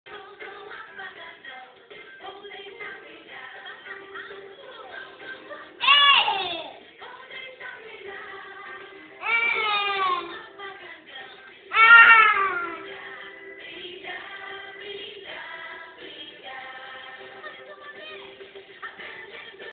Bebe Dançando e Cantando | Manuella 11 meses
Bebê de 11 meses dançando e cantando! Manuella, a pequena dançarina! Primeiro video do canal!